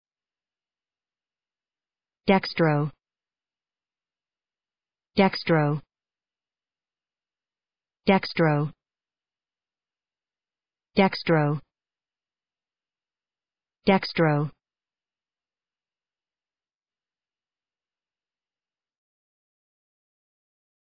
How to pronounce dextro
dextro American English pronunciation. How to pronounce dextro correctly. How to say dextro in proper American English.